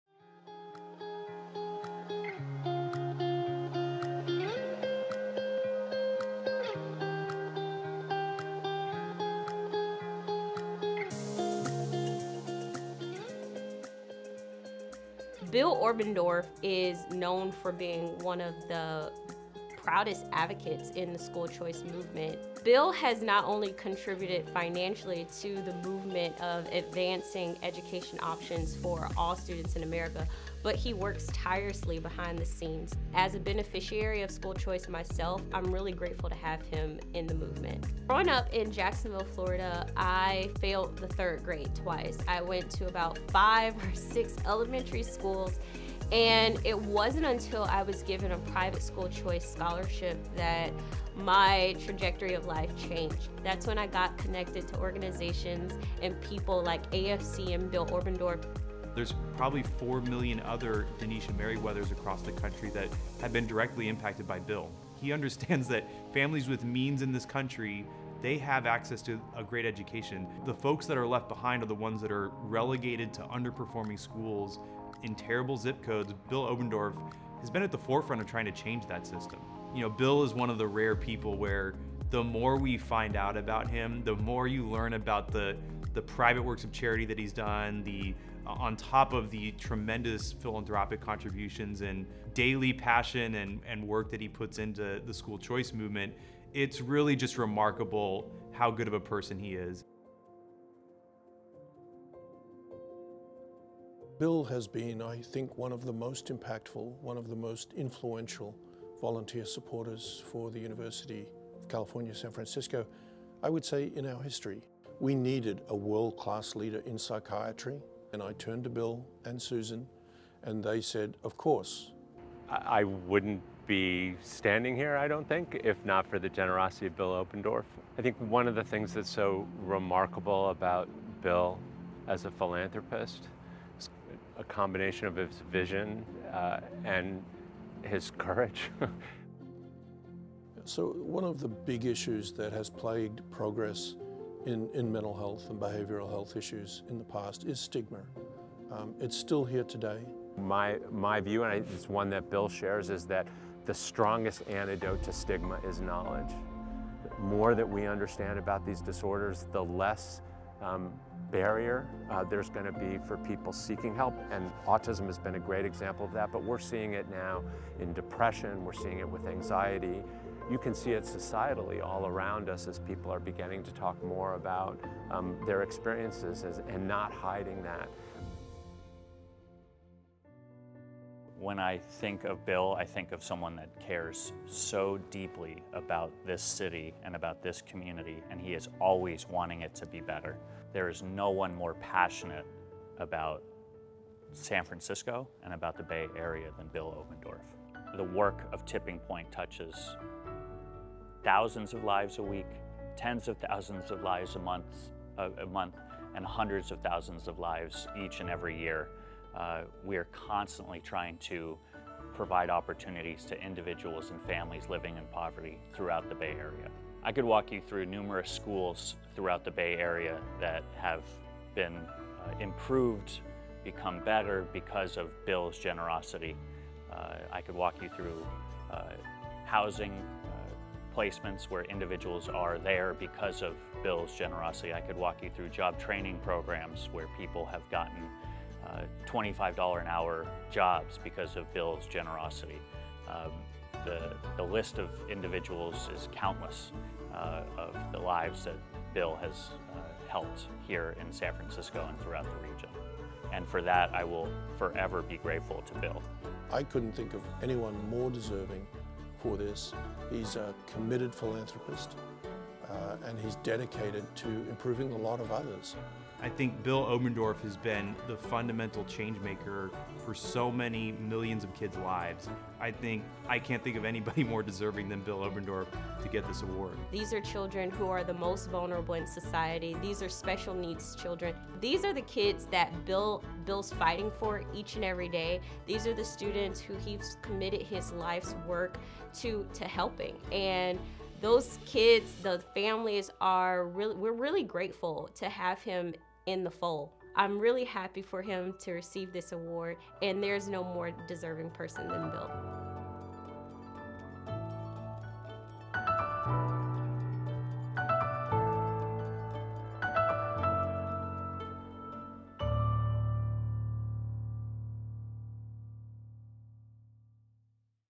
Meet 2021 Simon-DeVos Prize Winner Bill Oberndorf
Philanthropy Roundtable is honored to present investor and philanthropist William E. “Bill” Oberndorf with the 2021 Simon-DeVos Prize for Philanthropic Leadership, an award that recognizes a living philanthropist who has shown exemplary leadership through charitable giving. This video features just a few of the causes Oberndorf has championed: school choice, mental health research and poverty alleviation. Watch to learn more about the lives he has touched through his work with organizations like the American Federation for Children, Tipping Point Community and the University of California, San Francisco.